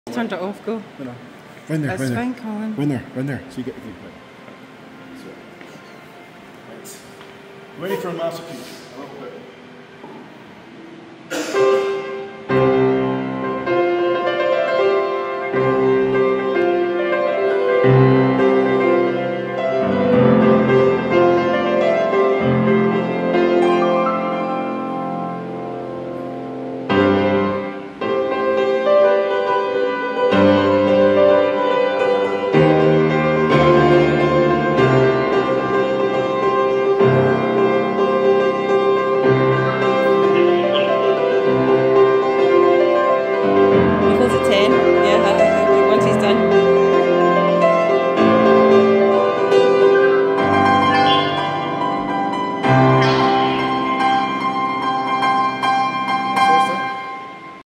Impromptu nightime piano solo "concert" at 122nd Floor of Seoul Sky before getting chucked out.
We were the last ones to leave. The place closes at 10pm, however this was just before 9:50pm and plenty of time for the elevator down.  Amazing views of Seoul at night. I havnt played piano for a long time so please forgive the mis-keys and bum notes lol.